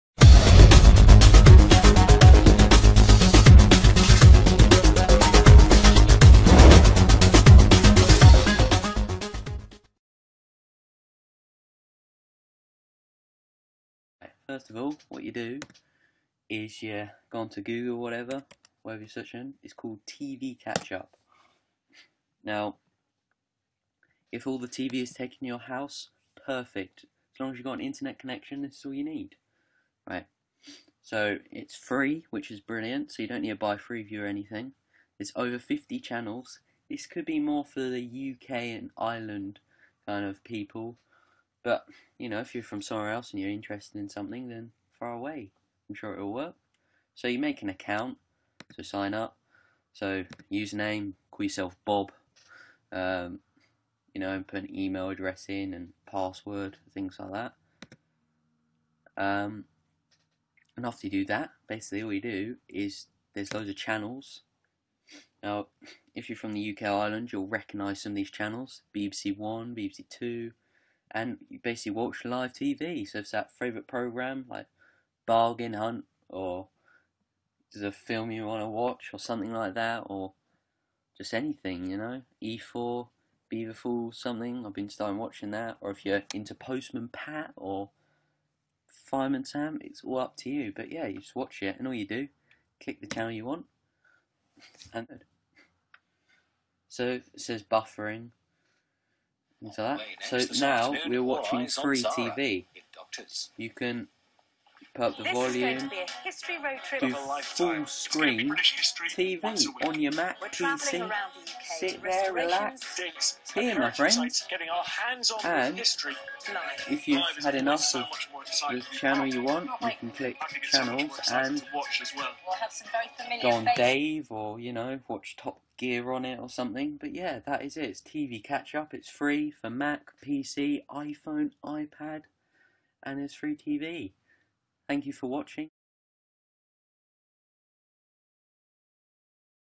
How To Watch FREE live UK TV Online
This is me showing you how to watch free tv on your PC, Mac, iPad or iPhone.

Go on to this site:

Then sign up for free by making a username and password. Thats it and there are over 50 channels you can watch for free. For example BBC, Film 4 and E4 etc.

If you have any questions please do not hesitate to ask.